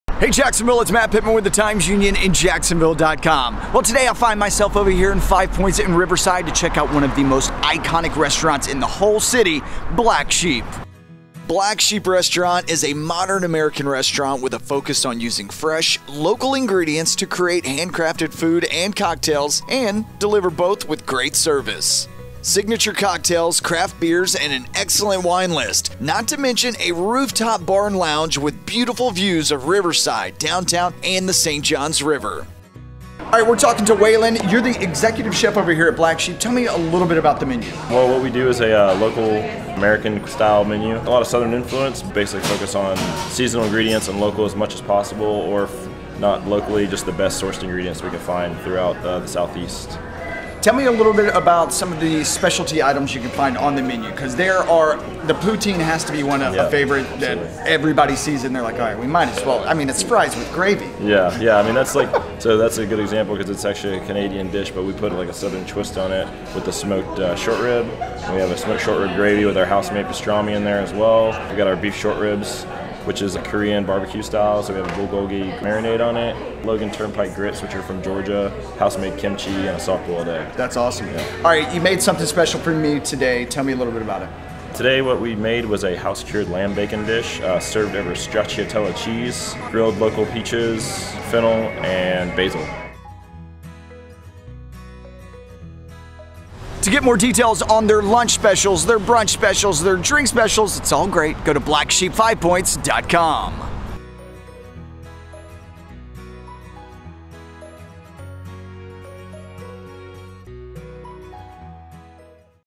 Discover Jacksonville | Start Here | Riverside | Black Sheep Restaurant
SPONSORED Discover Jacksonville's Start Here hosted video tours feature the iconic destinations unique to the neighborhoods of Jacksonville, and the places that make our city an amazing place to call home. In this episode, we highlight Black Sheep Restaurant in Riverside. Hosted by Matt Pittman.